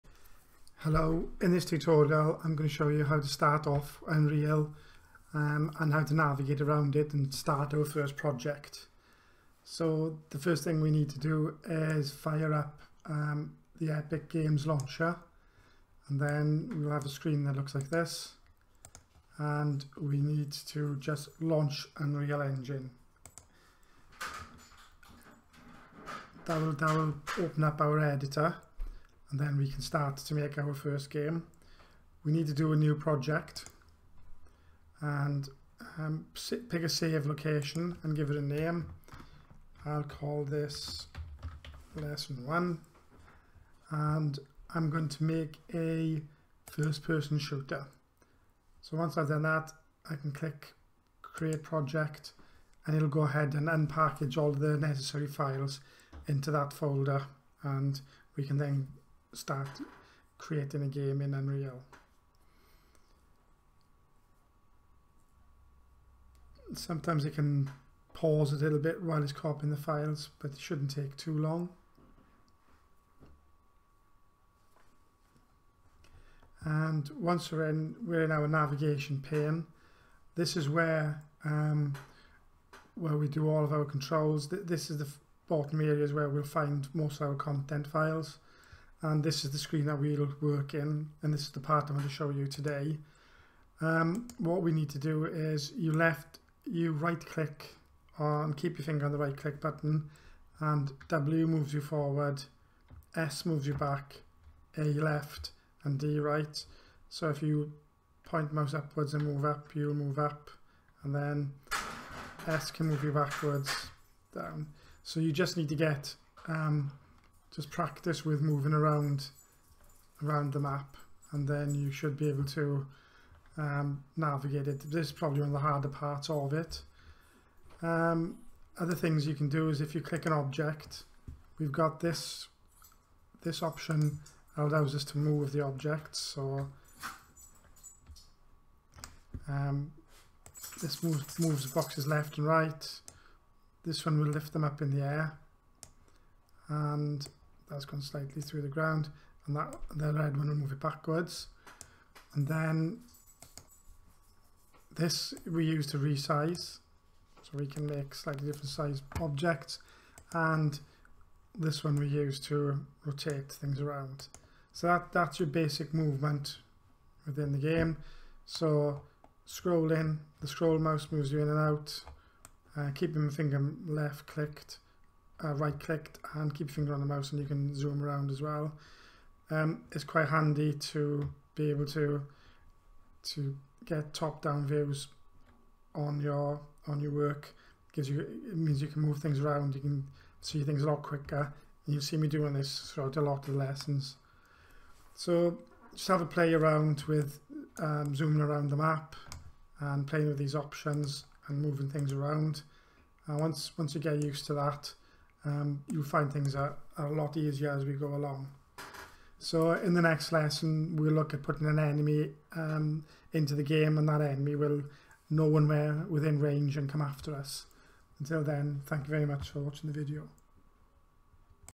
Unreal Engine 4 tutorial #1 Starting off
This is the first in a series of lessons, we will learn how to navigate around the Unreal Engine screens.